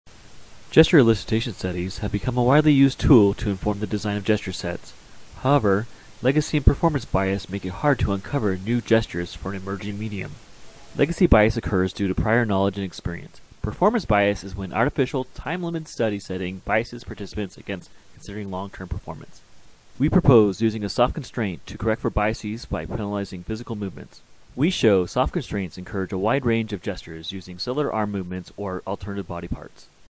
Soft-Constraints to Reduce Legacy and Performance Bias to Elicit Whole-body Gestures with Low ...
Soft-Constraints to Reduce Legacy and Performance Bias to Elicit Whole-body Gestures with Low Arm Fatigue
Jaime Ruiz, Daniel Vogel

Abstract:
Participant biases can influence proposed gestures in elicitation studies. There is a legacy bias from previous experience with, or even knowledge of, existing input devices, interfaces, and technologies. There is also a performance bias, where the artificial study setting does not encourage consideration of long-term aspects such as fatigue. These biases make it especially difficult to uncover gestures appropriate for whole-body gestural input. We propose using soft constraints to correct for legacy and performance biases by penalizing physical movements. We use wrist weights as a soft constraint to elicit whole-body gestures with low arm fatigue. We show soft constraints encourage a wider range of gestures using subtler arm movements or alternate body parts and lower consumed endurance for arm movements.